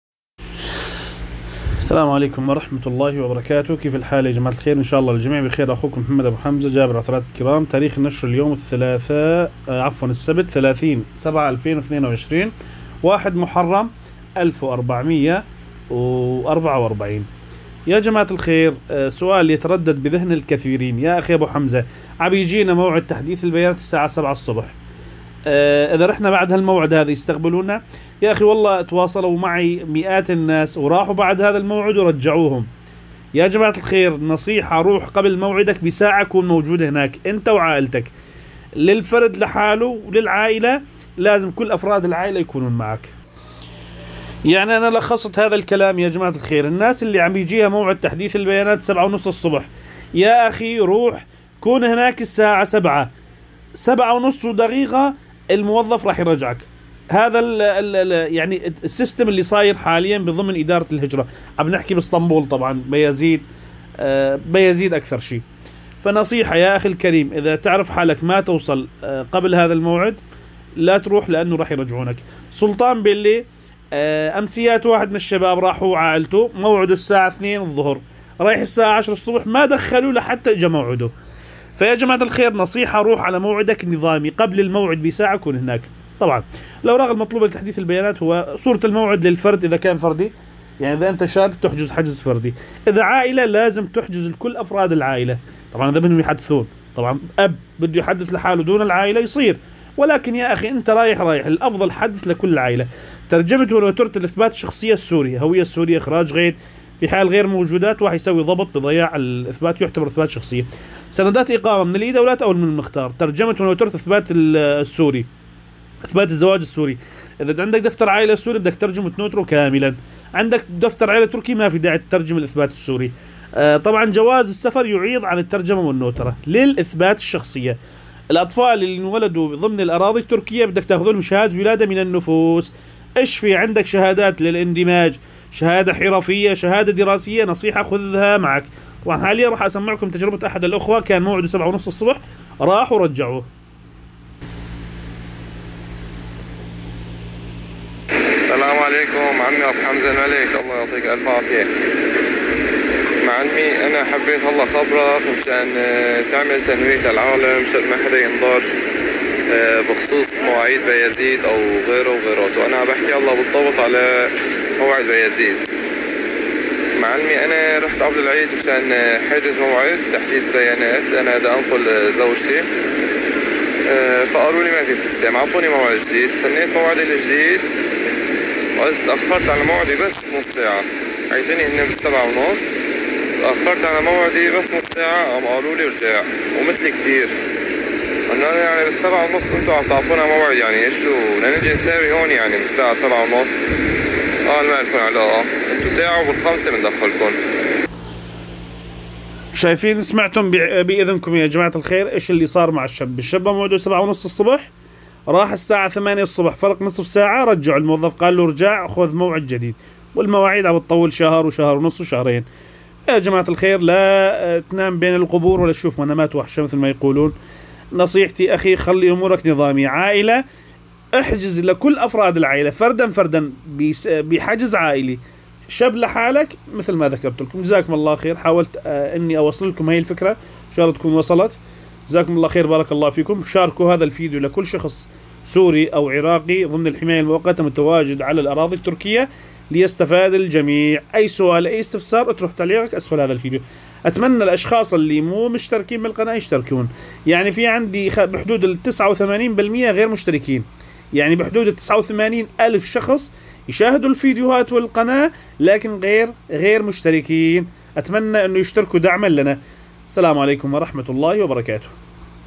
اللي عم يجي موعد تحديث لبيانات تبعه7.30الصبح نصائح،تنويهات وتجربة أحد الإخوة رجعوا لان تأخر نصف ساعة
الناس اللي عم يجيها موعد تحديث البيانات
7.30الصبح اذا رحنا بعد هي ساعة الجواب تابع الفيديو للاخير،
الاوراق المطلوبة لتحديث البيانات
●صورة الموعد فردي اذا كان فرد وعائلي للعائله.
●ترجمة ونوترة إثبات شخصية سوريا.
●سندات الاقامة من ال e.devlet او من المختار.
●ترجمة ونوترة إثبات زواج سوري.
●شهادات ولادة من النفوس للأطفال اللي نولدو بتركيا.
●شهادة الاندماج وايا شهادة حرفية أو دراسية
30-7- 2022